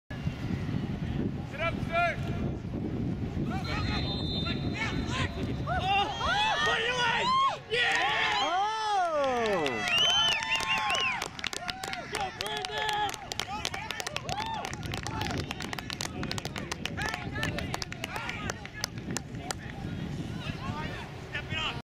Drew v Stuart Hall - Sept. 21 2011
Drew goal by Brandon N at approx. 24 min.

Final: 1-1